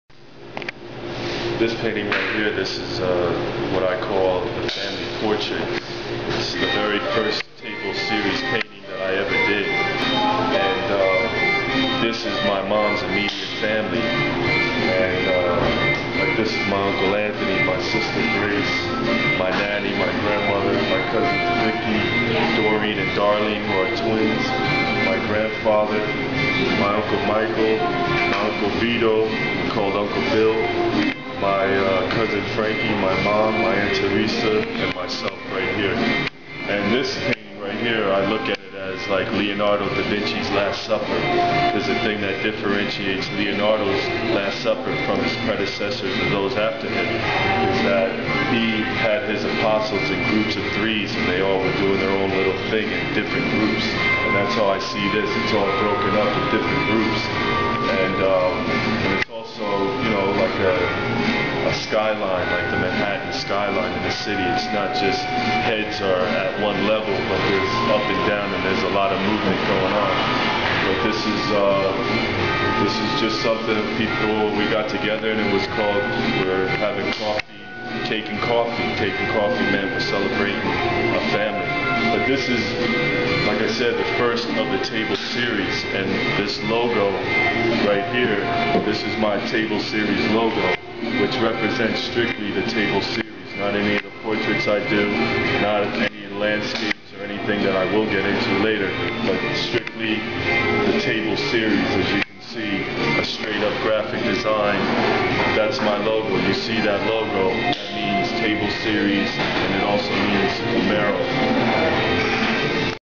Miceal Fumaro Jersey Artist At LITM in Downtown Jersey City
Heres Micheal Fumaro explaining his Table series.